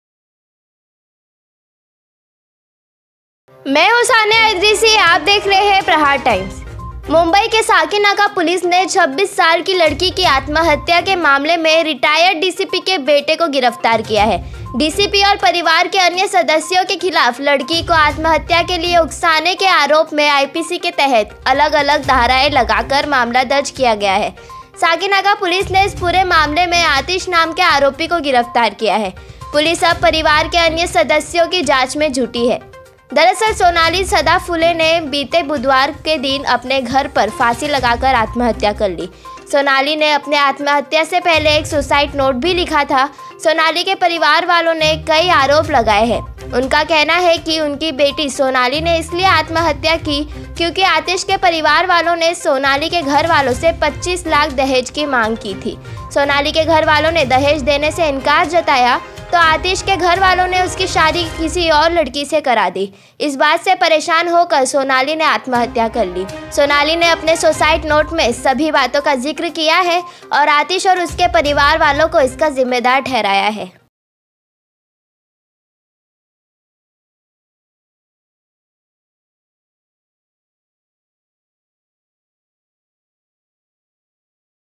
26 साल की महिला ने की खुदखुशी रिटायर DCP के परिवार वालो को ठराया ज़िम्मेदार||Saniya Idrisi
26 साल की महिला ने की खुदखुशी
रिटायर DCP के परिवार वालो को ठराया ज़िम्मेदार||Saniya Idrisi